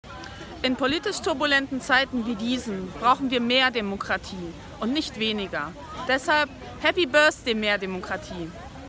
30 Jahre Mehr Demokratie: Geburtstagsgrüße von Katharina Nocun
Katharina Nocun, Bürgerrechtlerin und Netzaktivistin, gratuliert Mehr Demokratie zum 30-jährigen Jubiläum. Unser gemeinnützige Verein war am 12. Juli 1988 zunächst als „Initiative Demokratie Entwickeln – IDEE“ gegründet worden und hat sich später in Mehr Demokratie umbenannt. Hier unsere bisherige Bilanz